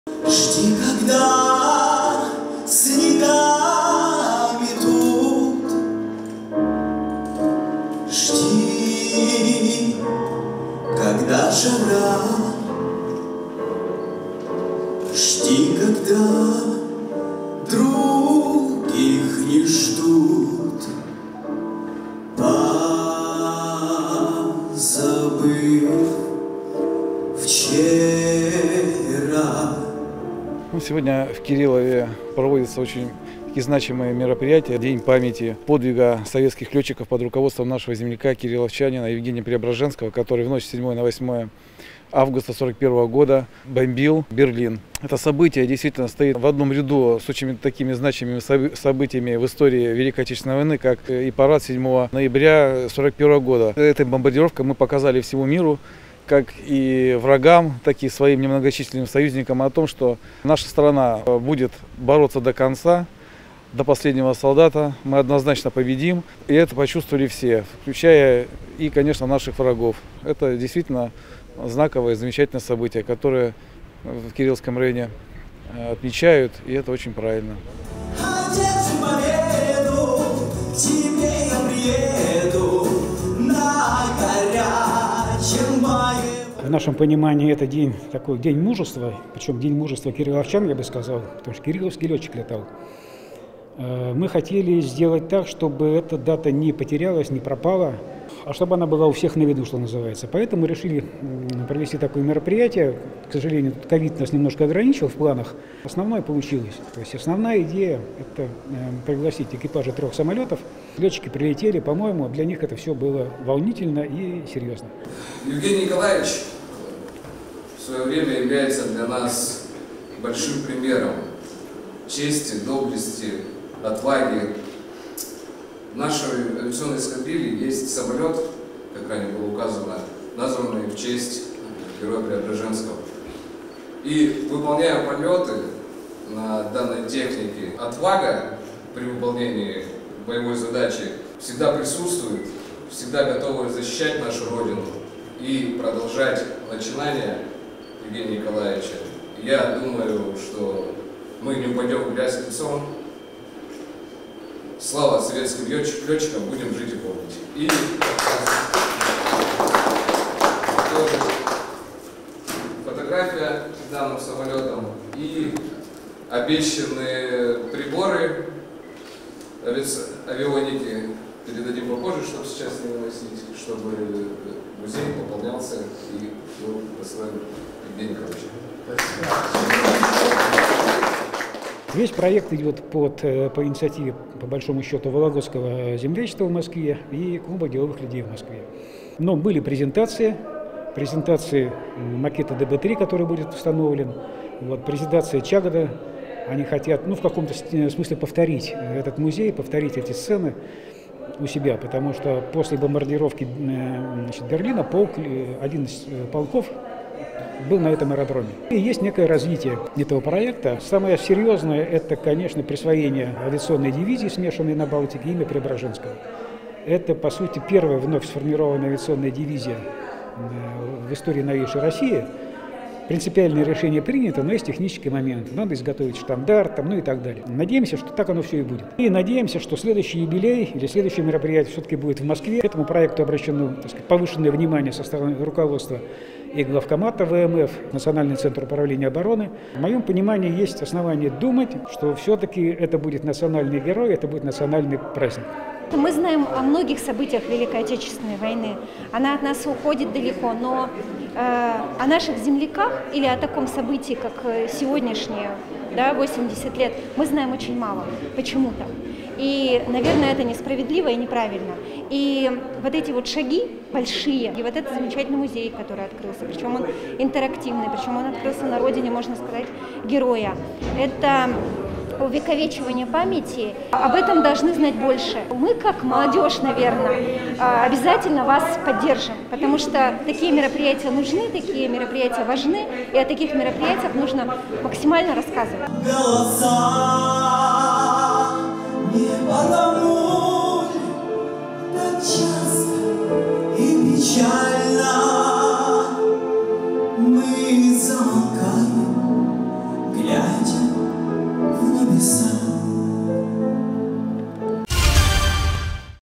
80-летие подвига летчика Преображенского отметили в Кириллове
Мероприятия посвящены годовщине первой бомбардировки Берлина в августе 1941 года авиационной группой под руководством уроженца Кирилловского района, Героя Советского Союза Евгения Преображенского. В Кириллов 8 августа прибыли экипажи самолетов, носящих имя Героя – военного ТУ-142 из Кипелово, пассажирского лайнера Sukhoi Superjet 100 авиакомпании «Северсталь» и борта «Бе-200» МЧС России, приспособленного для тушения пожаров. В Музее Евгения Николаевича Преображенского прошла основная торжественная часть мероприятия и экскурсия по экспозиции для всех желающих. Также участники мероприятия возложили цветы к бюсту героя-земляка.